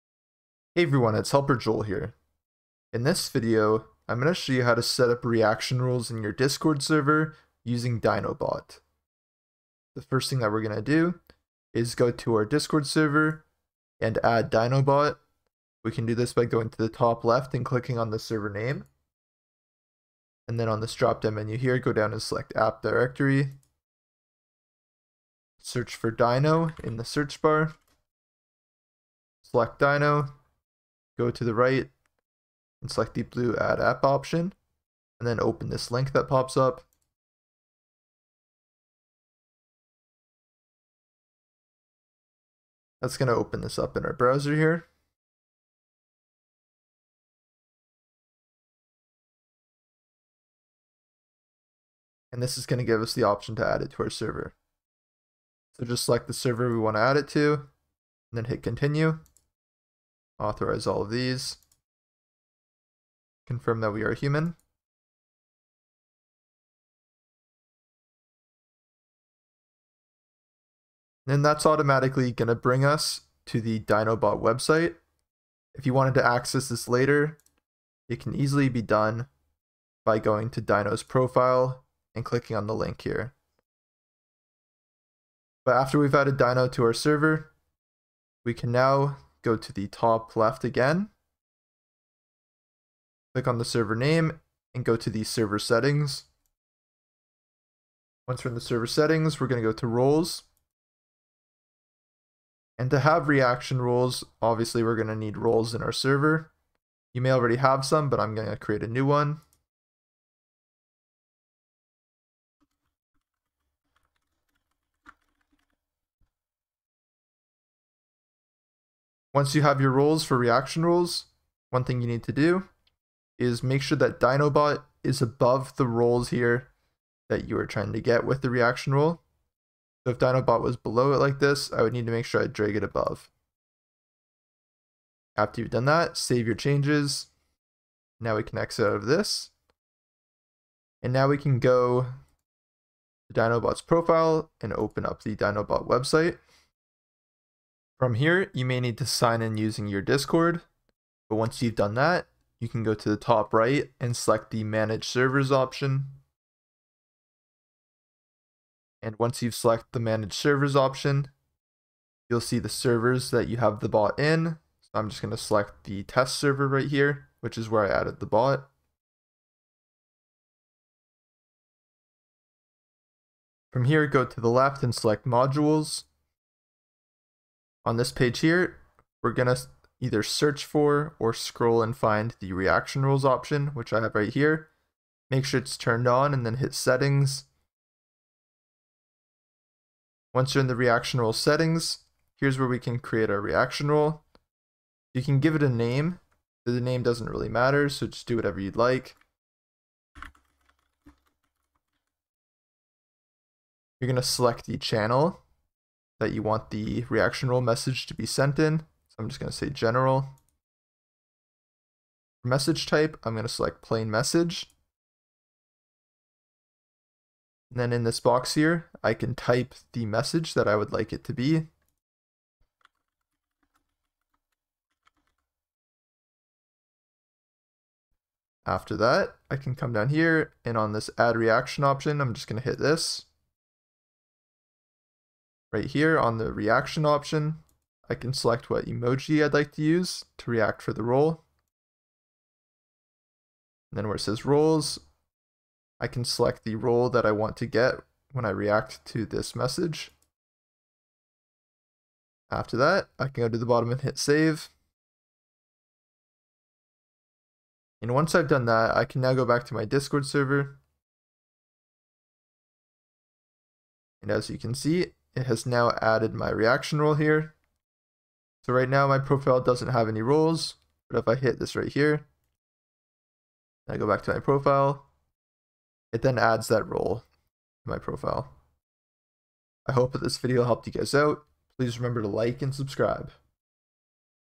How To Setup Reaction Roles Using Dyno Bot
This video shows you how to setup auto roles in your Discord server using Dyno bot. By following along this tutorial, setting up auto roles in your server will be easy.